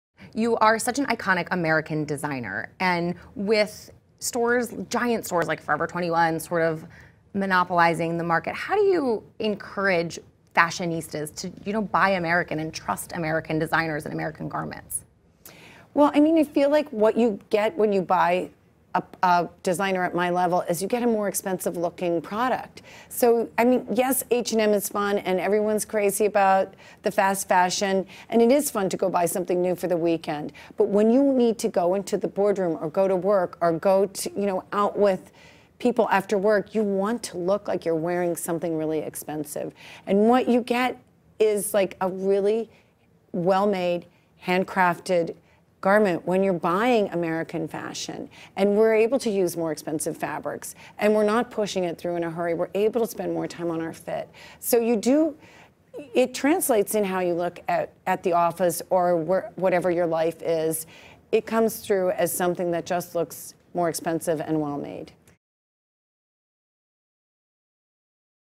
Nanette Lepore Talks Quality | HPL
More Below

Nanette Lepore joins Caitlyn to talk about why cheaper and mass-produced clothes don't cut it when it comes to fashion.